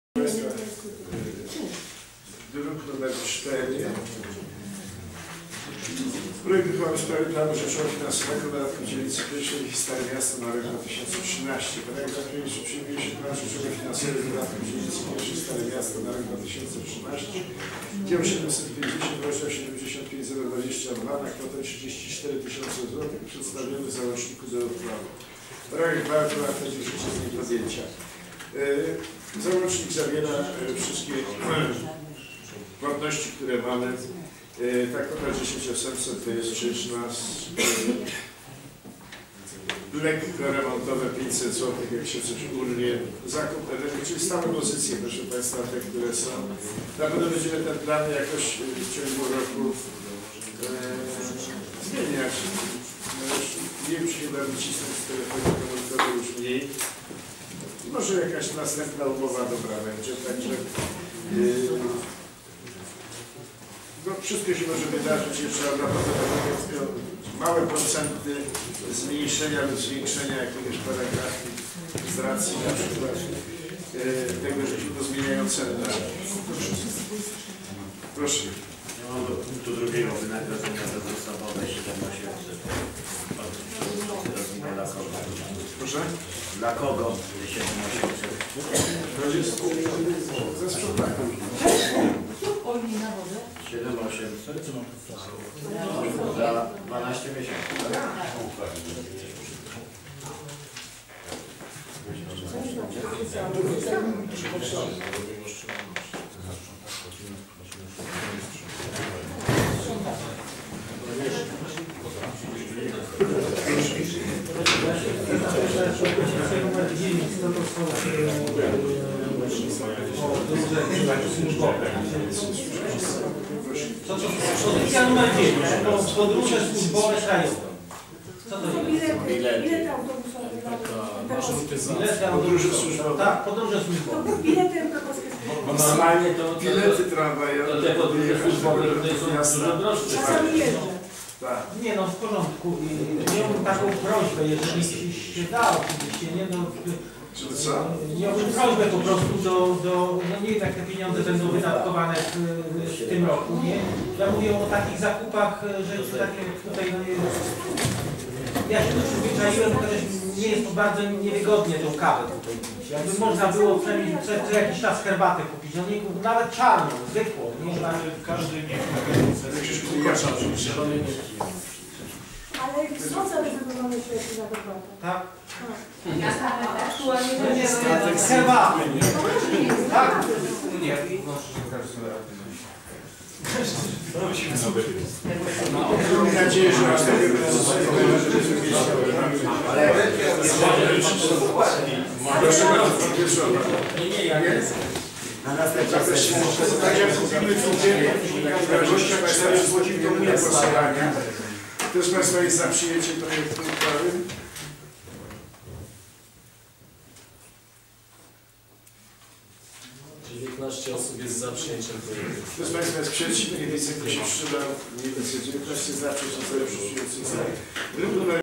RD I 24 01 2013 6 plan finansowo rzeczowy na rok 2013
Sesja XXVIII cz.6
Przyjęcie planu finansowo rzeczowego dzielnicy I na rok 2013.